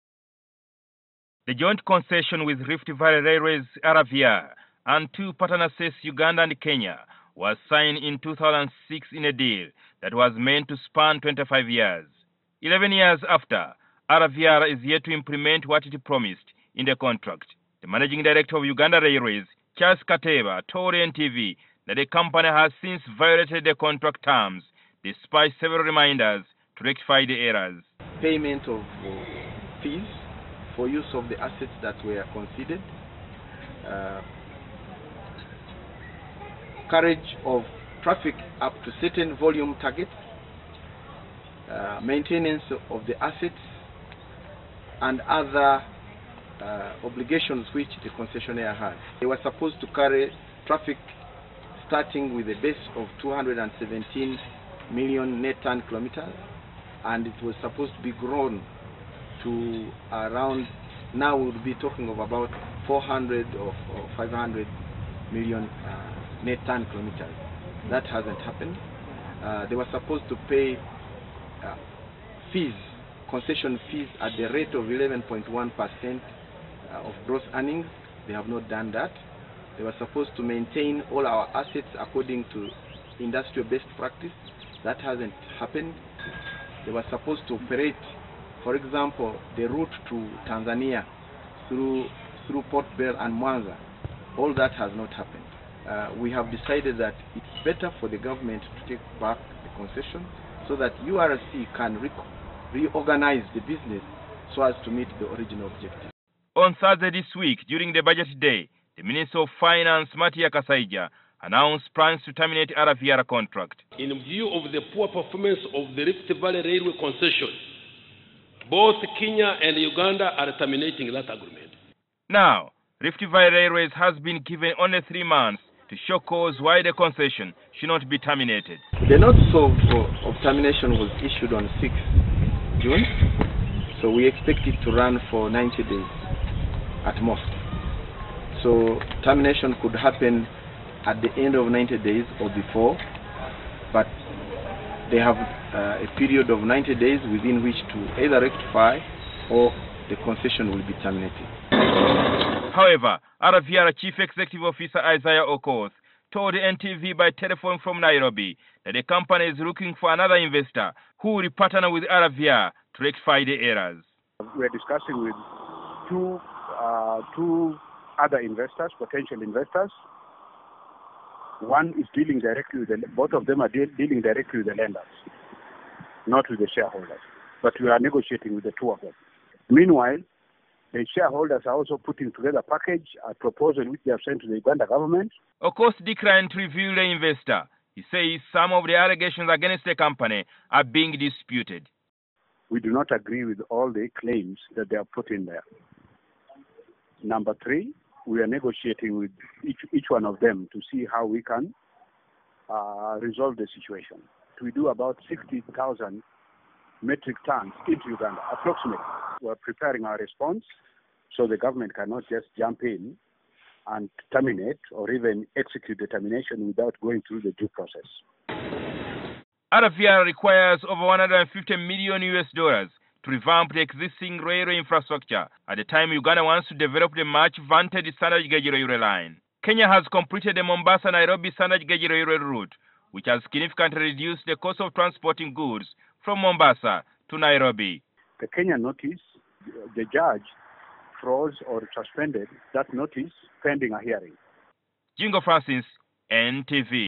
Rift Valley Railways faces contract termination
Rift valley Railways (RVR), the company contracted by government to run railways services in Uganda, has been handed a three month ultimatum to implement the contract terms or else face a termination of its concession by the two partner states Uganda and Kenya.

The joint concession between RVR and two partner states, Uganda and Kenya, was signed in 2006 in a deal that was meant to span 25 years. Eleven years later, RVR is yet to implement what it promised in the contract.

The Managing Director of Uganda Railways, Charles Kateeba, told NTV that the company has since violated the contract terms despite several reminders to rectify the errors.

On Thursday this week during the budget day, the minister of finance Matia Kasaija announced plans to terminate the RVR contract. Now, RVR has been given only three months to show cause why the concession should not be terminated.